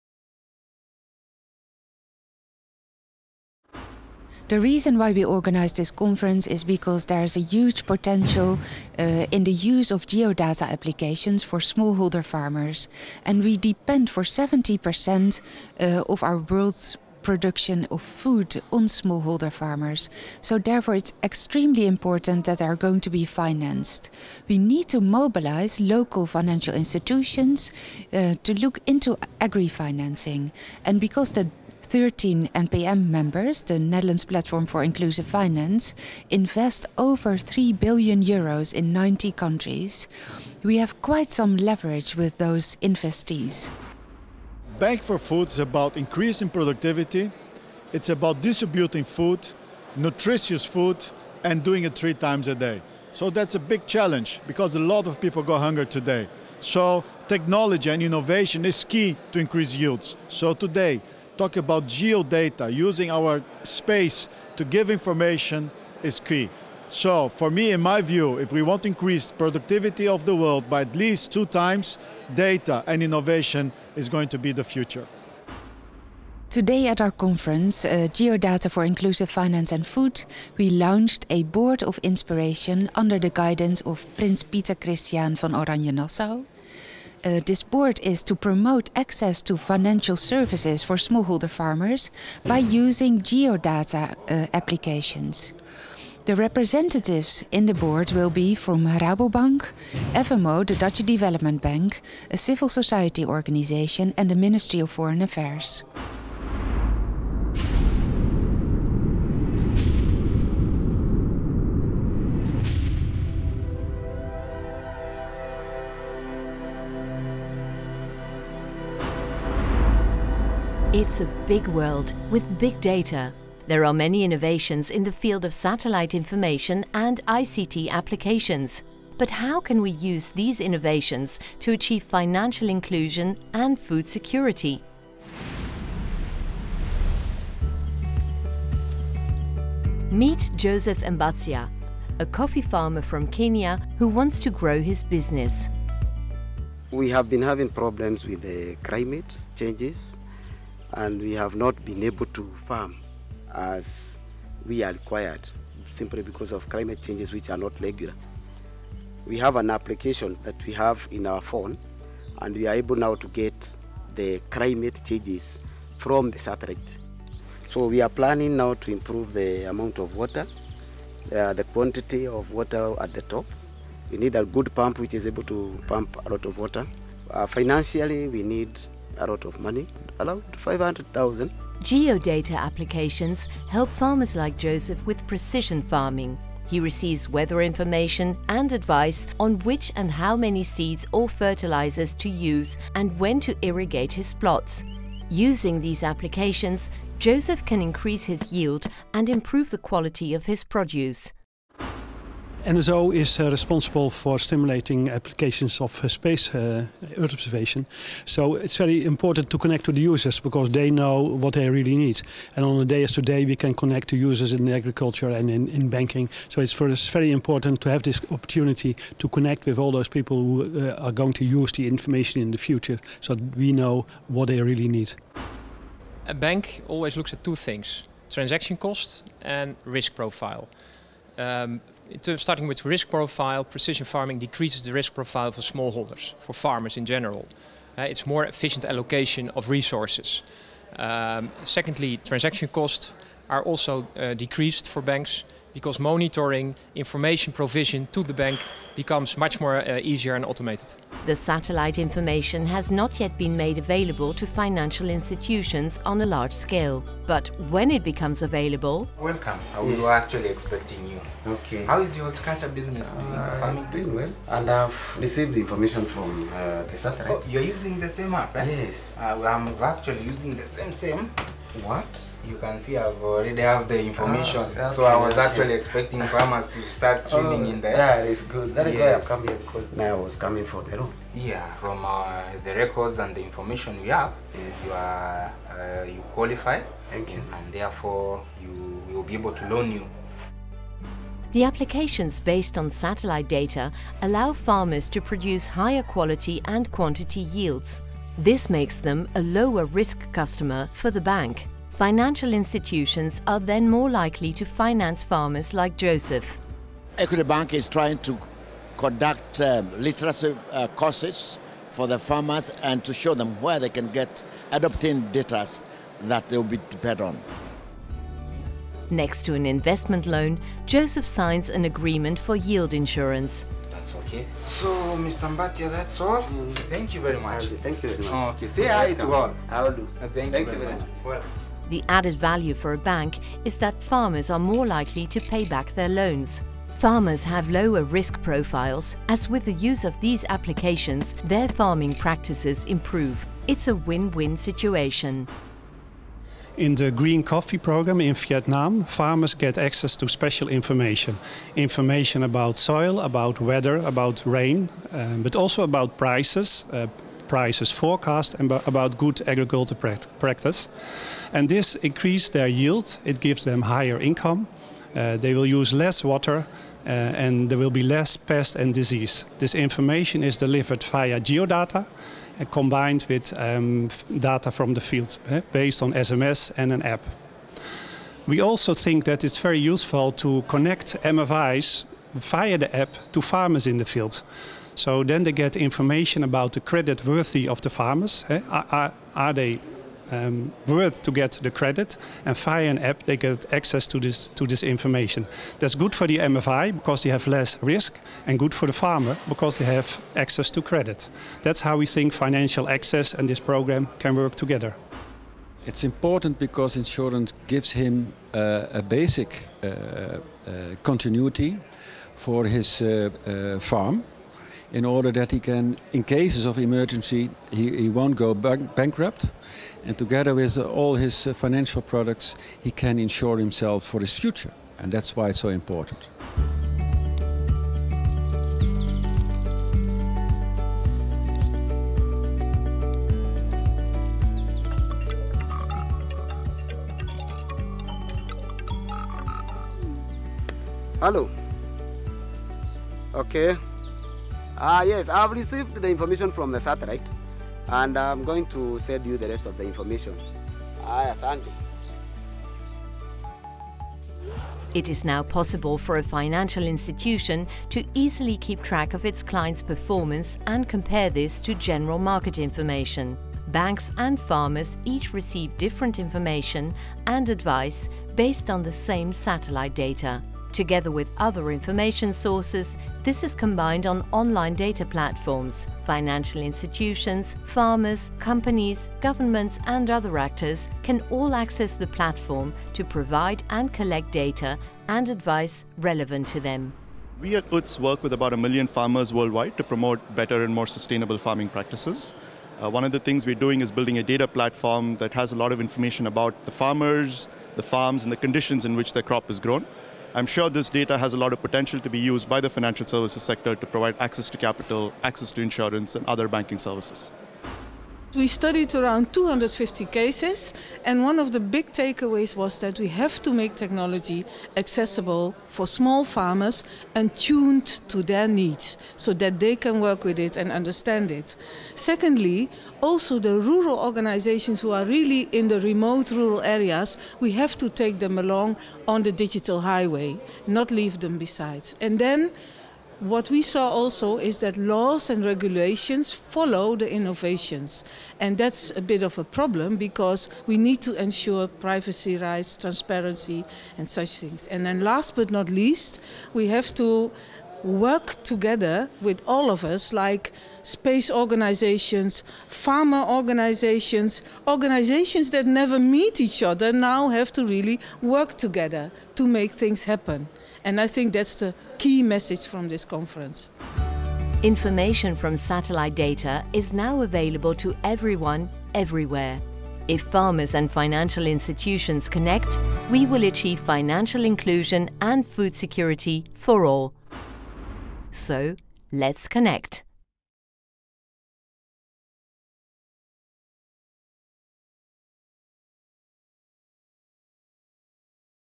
Geodata for Inclusive Finance and Food
On 16 February 2017, NpM Platform for Inclusive Finance, the Netherlands Space Office and Rabobank Foundation organised a conference on Geodata for Inclusive Finance and Food.